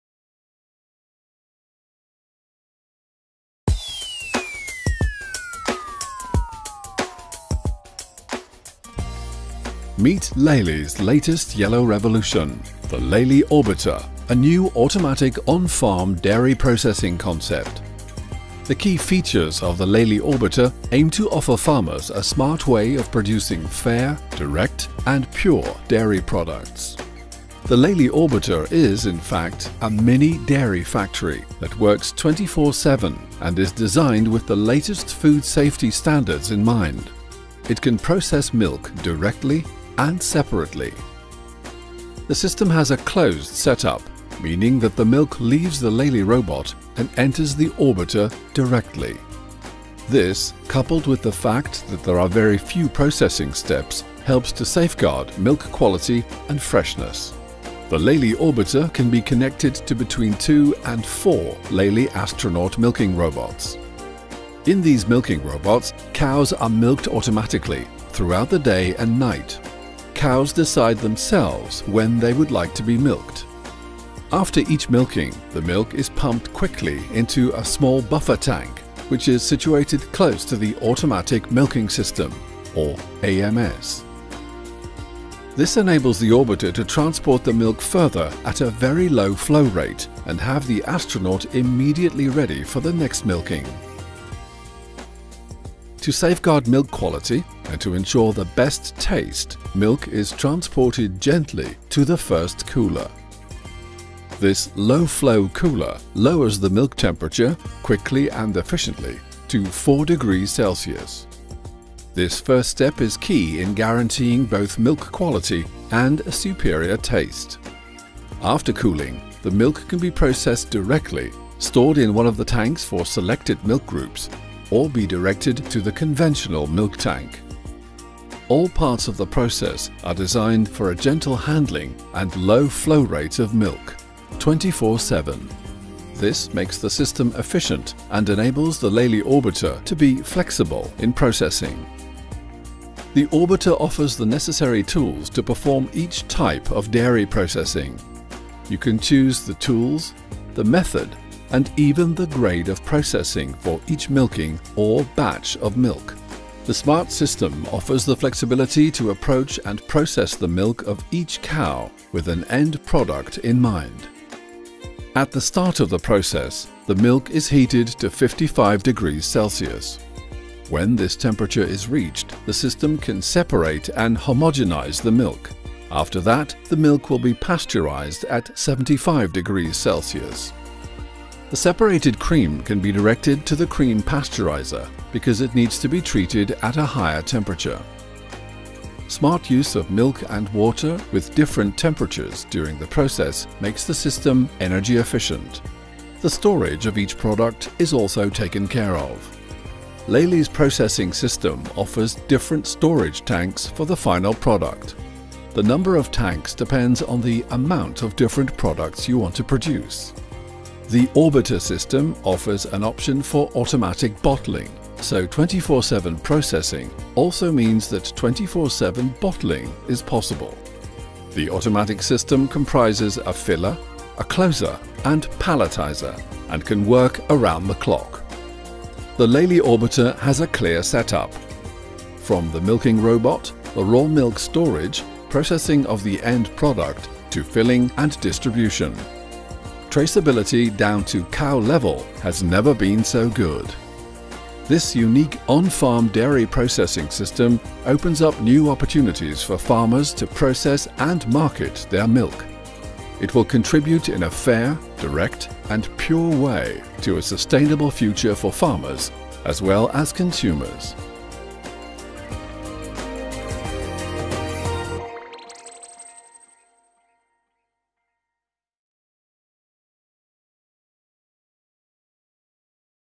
Lely Orbiter Nasıl çalışır?
Süt sığırcılığının geleceği olacak çiftlik içerisinde sütü işleme sistemimiz tanıtıldı.